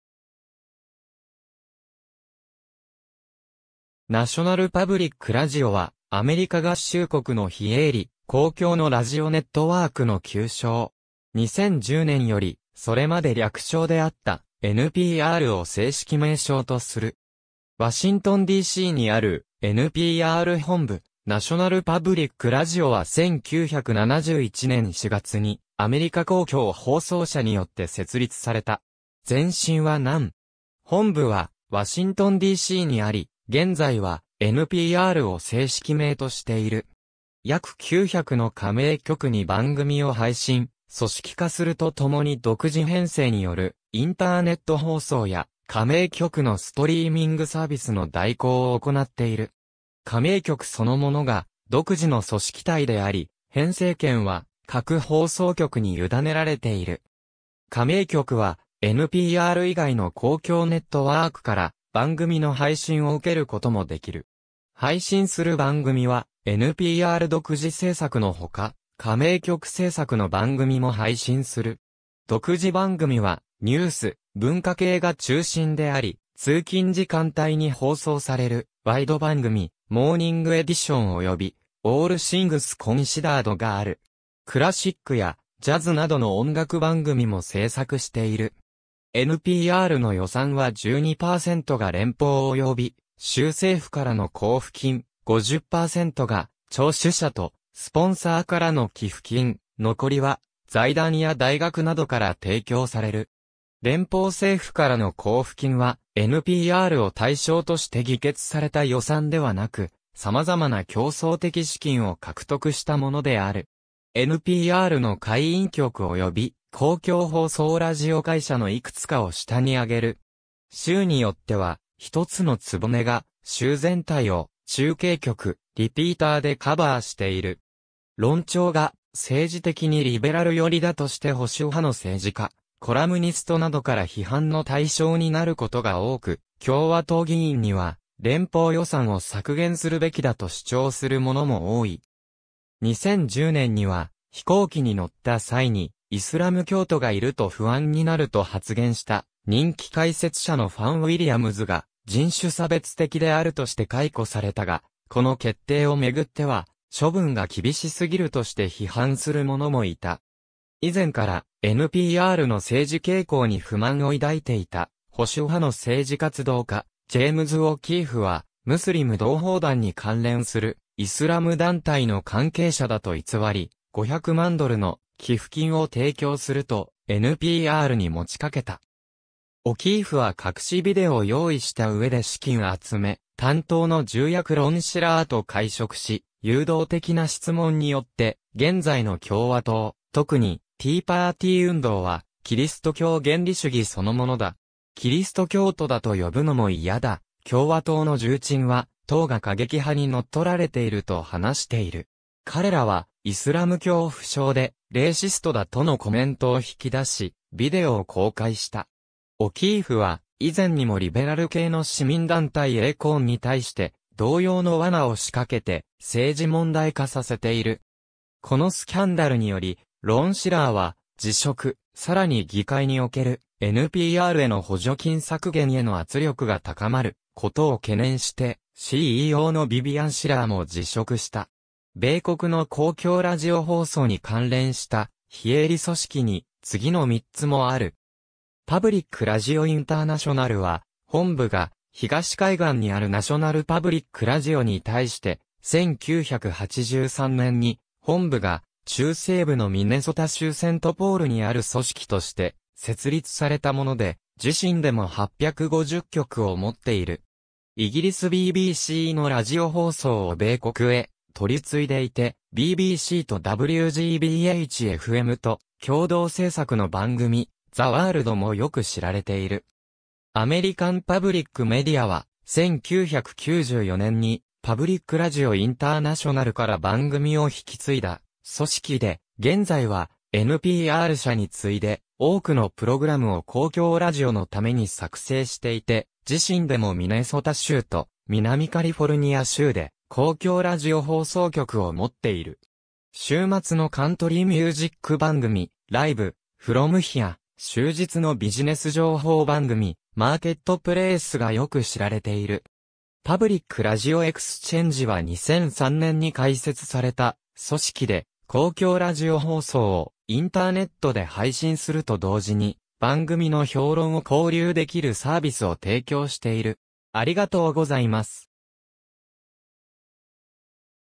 ナショナル・パブリック・ラジオ
ナショナル・パブリック・ラジオ（米公共ラジオ局などとも、アメリカ英語: National Public Radio）は、アメリカ合衆国の非営利・公共のラジオネットワークの旧称。
2010年より、それまで略称であったNPRを正式名称とする。
ワシントンD.C.にあるNPR本部 ナショナル・パブリック・ラジオ（NPR）は1971年4月にアメリカ公共放送社 (CPB)によって設立された。
前身はNERN(National Educational Radio Network、全国教育ラジオネット)。
本部はワシントンD.C.にあり、現在はNPRを正式名としている。
約900の加盟局に番組を配信、組織化するとともに独自編成によるインターネット放送や、加盟局のストリーミングサービスの代行を行っている。
加盟局そのものが独自の組織体であり、編成権は各放送局に委ねられている。
加盟局はNPR以外の公共ネットワークから番組の配信を受ける事も出来る（日本語でいうクロスネット局）。
配信する番組はNPR独自制作のほか、加盟局制作の番組も配信する。
独自番組はニュース・文化系が中心であり、通勤時間帯に放送されるワイド番組『Morning Edition』（聴取者は全国で1400万人と言われる看板番組）及び『All Things Considered』がある。
クラシックやジャズなどの音楽番組も制作している。
NPRの予算は12%が連邦および州政府からの交付金、50%が聴取者とスポンサーからの寄付金、残りは財団や大学などから提供される。
連邦政府からの交付金はNPRを対象として議決された予算ではなく、様々な競争的資金を獲得したものである。
NPRの会員局および公共放送ラジオ会社のいくつかを下に挙げる。
州によっては、ひとつの局が州全体を中継局、リピーターでカバーしている。
論調が政治的にリベラル寄りだとして保守派の政治家、コラムニストなどから批判の対象になることが多く、共和党議員には連邦予算を削減するべきだと主張するものも多い。
2010年には「飛行機に乗った際にイスラム教徒がいると不安になる」と発言した人気解説者のファン・ウィリアムズが人種差別的であるとして解雇されたが、この決定をめぐっては処分が厳しすぎるとして批判する者もいた。
以前からNPRの政治傾向に不満を抱いていた保守派の政治活動家ジェームズ・オキーフは、ムスリム同胞団に関連するイスラム団体の関係者だと偽り、500万ドルの寄付金を提供するとNPRに持ちかけた。
オキーフは隠しビデオを用意したうえで資金集め担当の重役ロン・シラーと会食し、誘導的な質問によって「現在の共和党、特にティーパーティー運動はキリスト教原理主義そのものだ。キリスト教徒だと呼ぶのもいやだ」「共和党の重鎮は党が過激派に乗っ取られていると話している。彼らはイスラム恐怖症でレイシストだ」とのコメントを引き出し、ビデオを公開した。
オキーフは以前にもリベラル系の市民団体ACORNに対して同様の罠を仕掛けて政治問題化させている。
このスキャンダルによりロン・シラーは辞職、さらに議会におけるNPRへの補助金削減への圧力が高まることを懸念してCEOのビビアン・シラー（ロン・シラーと血縁関係はない）も辞職した。
米国の公共ラジオ放送に関連した非営利組織に、次の三つもある。
パブリック・ラジオ・インターナショナル（略称：PRI）は、本部が東海岸にあるナショナル・パブリック・ラジオ（NPR）に対して、1983年に本部が中西部のミネソタ州セントポールにある組織として設立されたもので、自身でも850局を持っている。
イギリスBBCのラジオ放送を米国へ取り次いでいて、BBCとWGBH-FMと共同制作の番組「ザ・ワールド」（The World）もよく知られている。
アメリカン・パブリック・メディア（略称：APM）（本社：ミネソタ州セントポール、略称：APM）は、1994年にPublic Radio International（当時の名称はAmerican Public Radio）から番組を引き継いだ組織で、現在はNPR社に次いで多くのプログラムを公共ラジオのために作成していて、自身でもミネソタ州と南カリフォルニア州で公共ラジオ放送局を持っている。
週末のカントリー・ミュージック番組「ライブ・フロム・ヒア」（Live from Here、旧称：プレーリー・ホーム・コンパニオン）、週日のビジネス情報番組「マーケットプレース」（Marketplac...